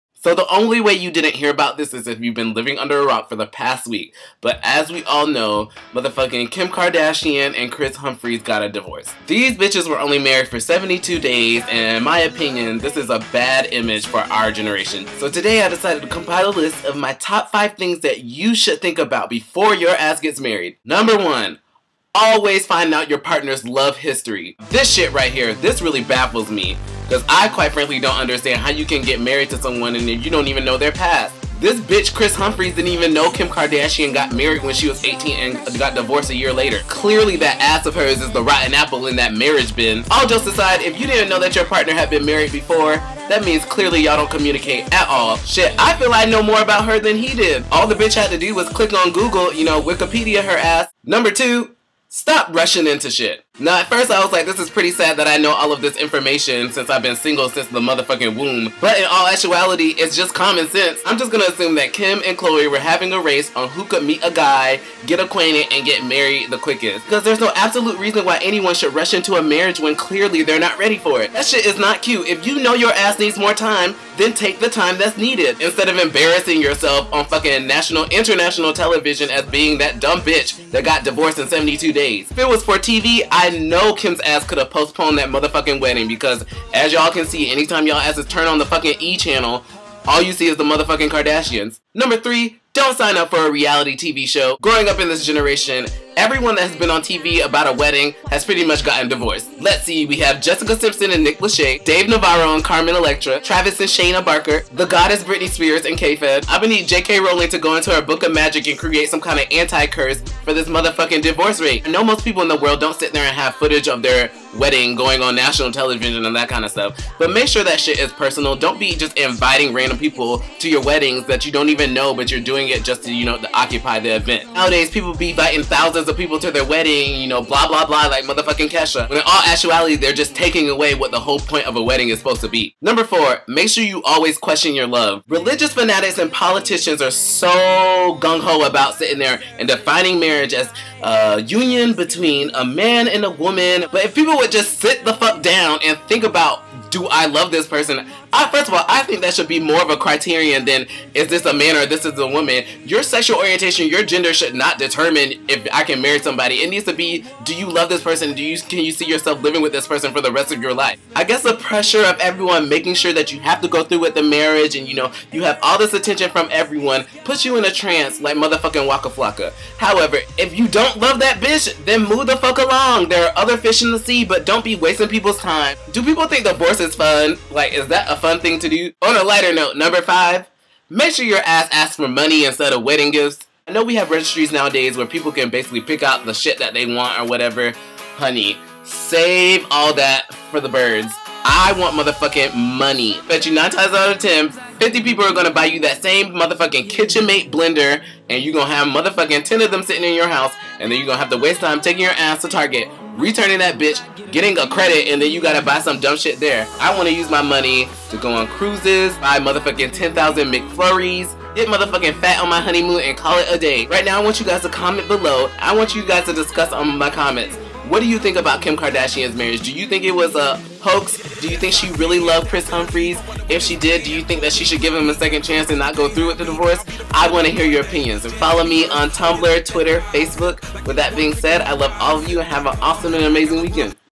Wedding Woes- Kardashian Edition
My personal opinion on the Kim K wedding and those thinking about marriage!